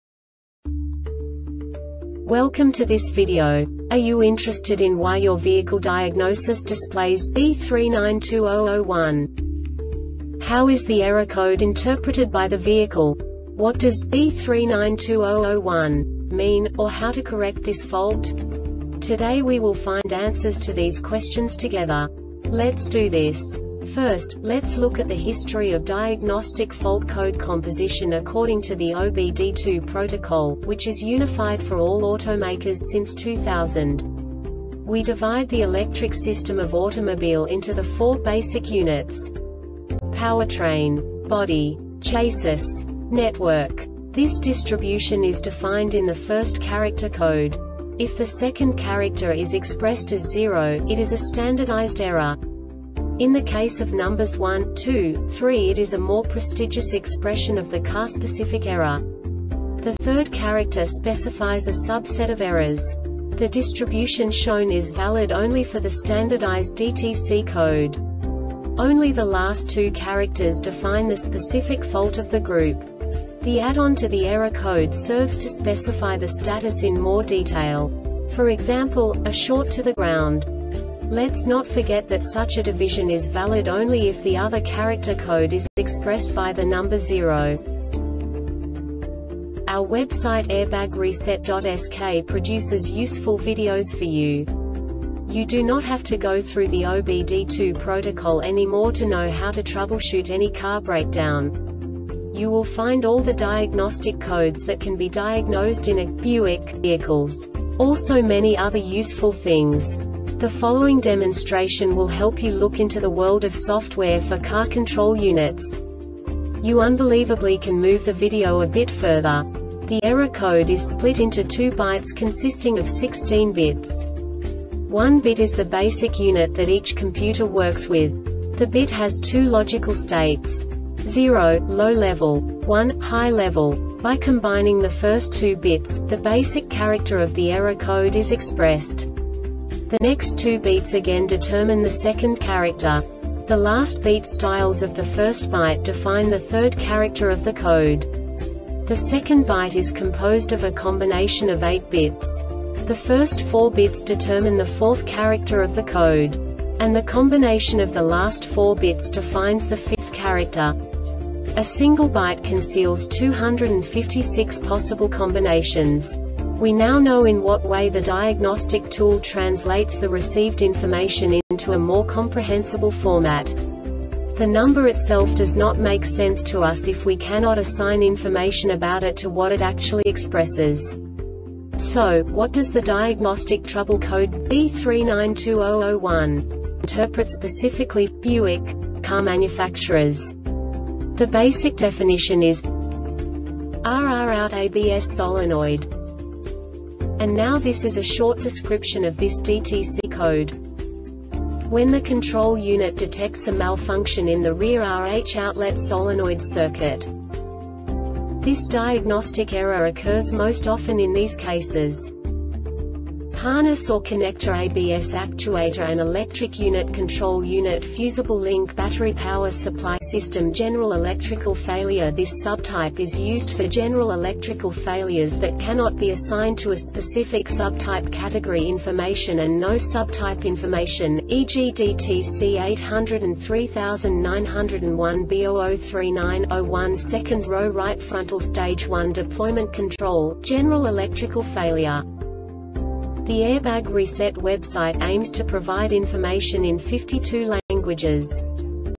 DTC Buick B3920-01 Short Explanation
Contents:
0:21 Basic DTC analysis according to OBD2 protocol standard.
1:48 Insight into programming
2:58 A diagnostically understandable description of the Diagnostic Trouble Code
3:05 Basic error definition

Make:
Buick
Code:
B3920 01
Definition:
RR Out ABS Solenoid
Description:
When the control unit detects a malfunction in the rear RH outlet solenoid circuit.
Cause:
 Harness or connectorABS actuator and electric unit (control unit)Fusible linkBattery power supply system
Failure Type: